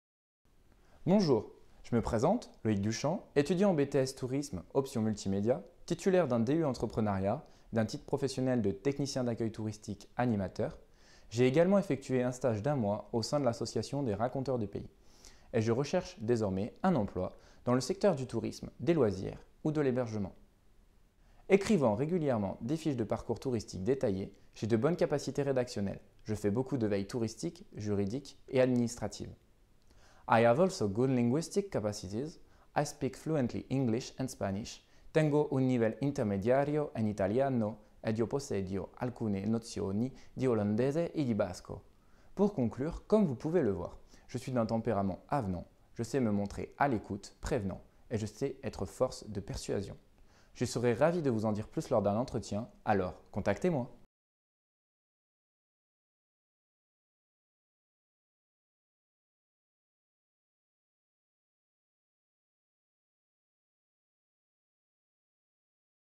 CV Video (Loïc Duchamp)
Réalisation de Curriculum Video sur Pau. Triplez vos chances d'obtenir un rendez-vous avec un recruteur grâce à un CV Video professionnel.

Production: Vidéo RVB Communication
Réalisation: Vanessa B.